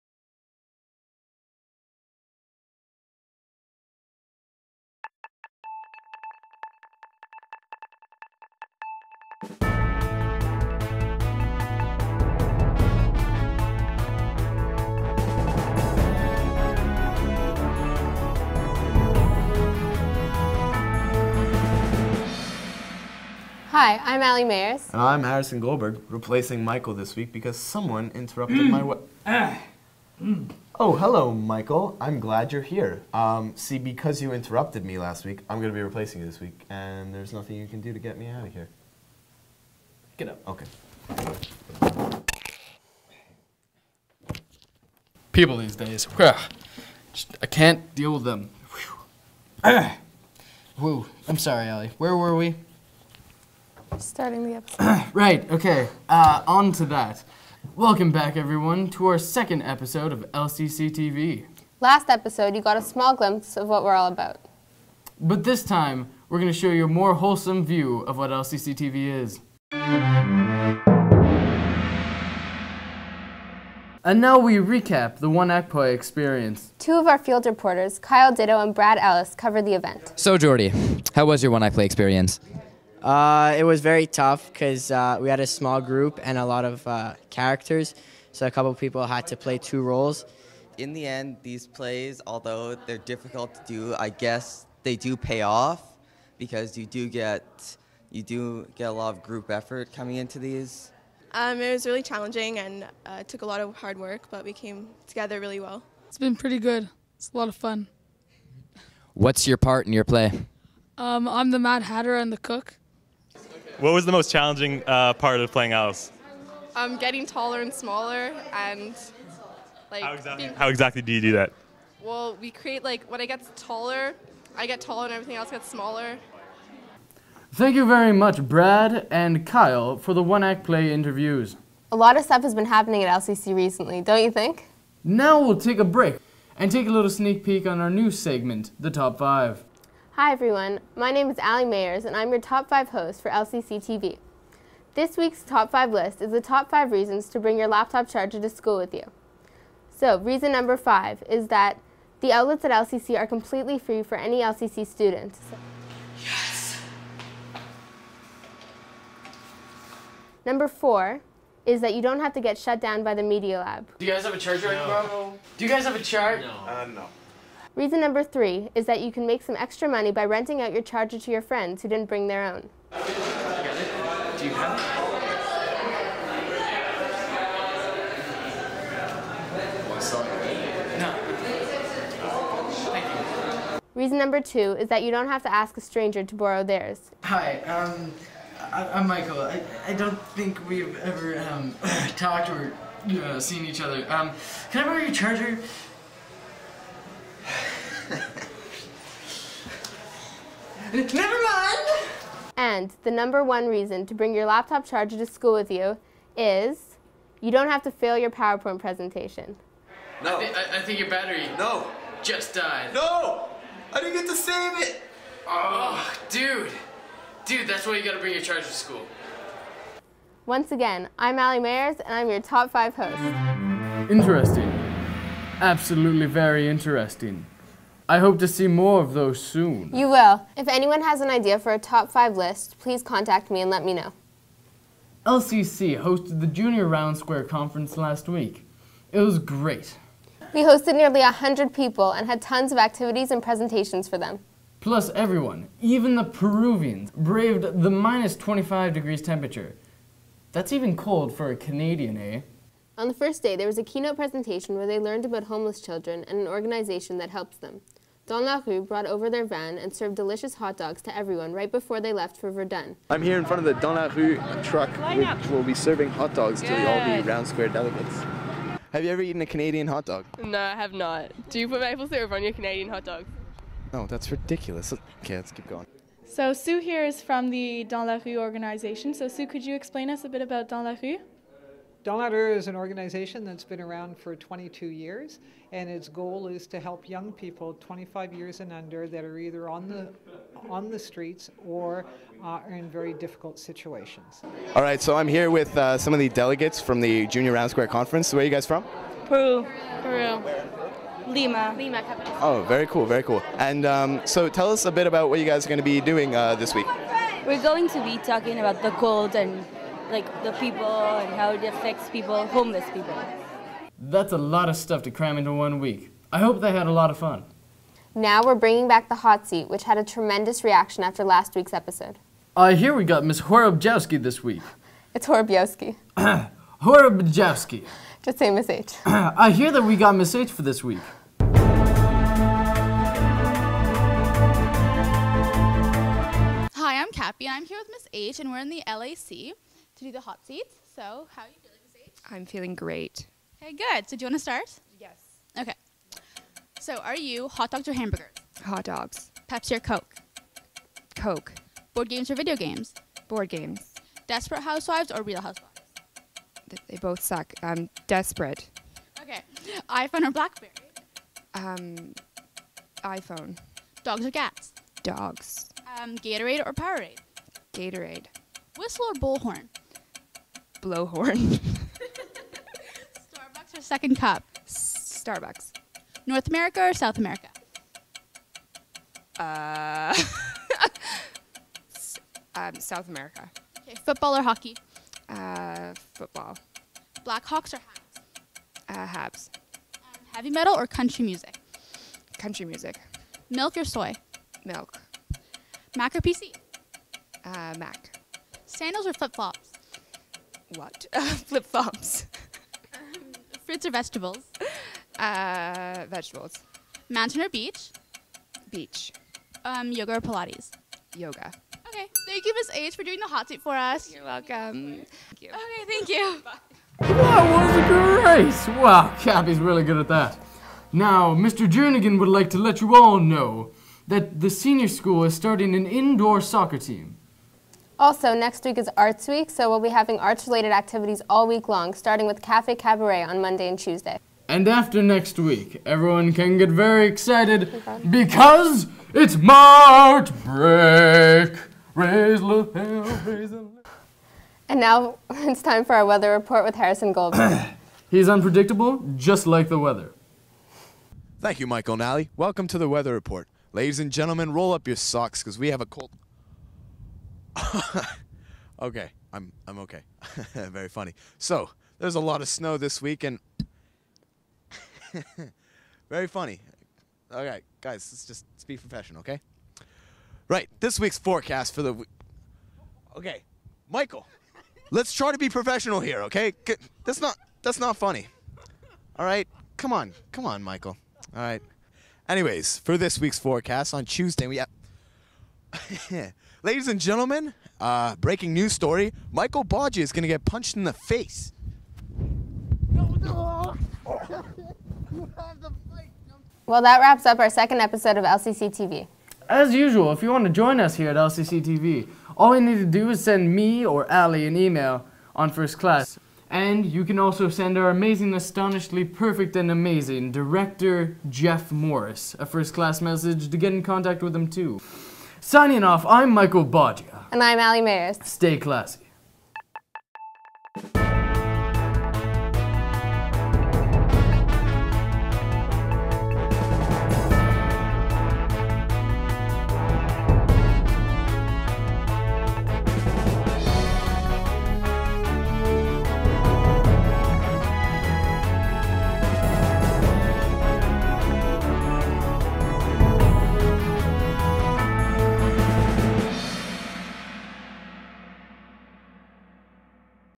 LCCTV - Episode 2
Michael and Allie are back for episode 2:

- One Act Play Recap by Brad and Kyle for the LCC Moment
- Top 5 reasons to bring your charger to school by Allie
- Junior Round Square Conference Recap by Harrison and other LCC TV crew
- Hot Seat #2 with Ms. H by Cappy